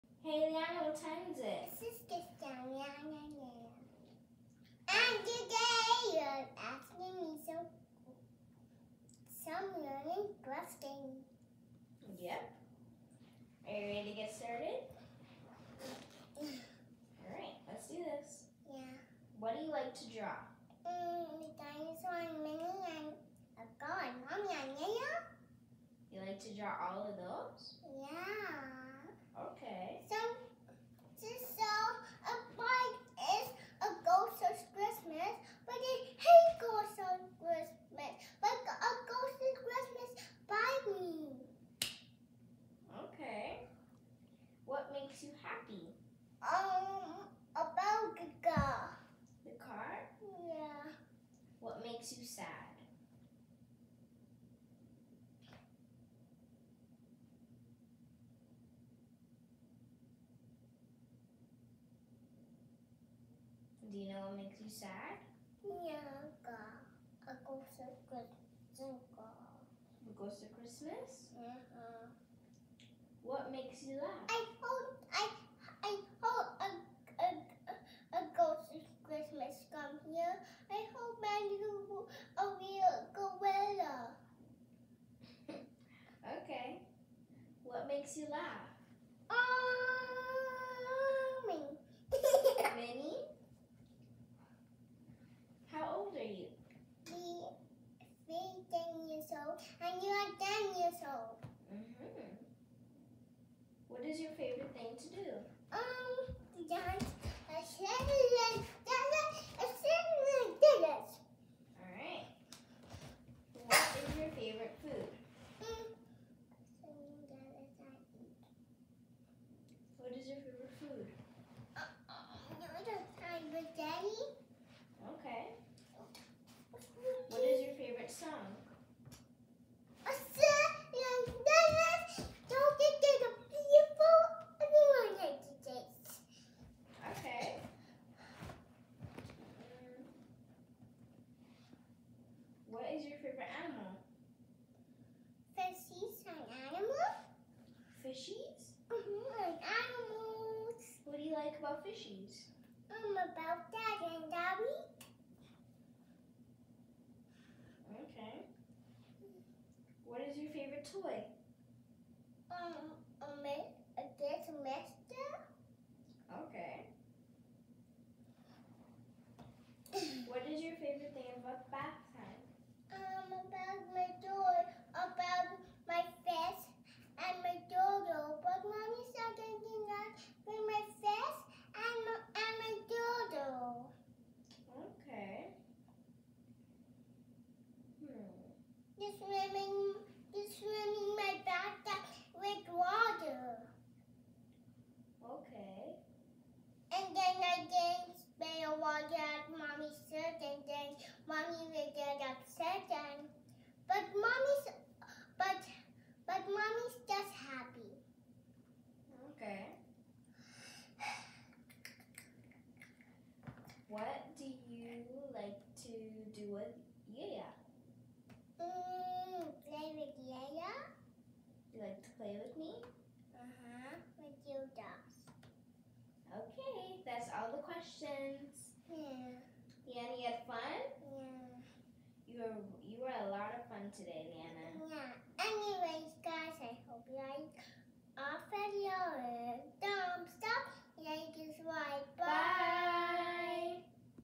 Lianna’s Big Interview Part 1
Hi guys. today I decided to interview my 3 year old sister. She sure didn’t disappoint. Hopefully you get to know her a little more.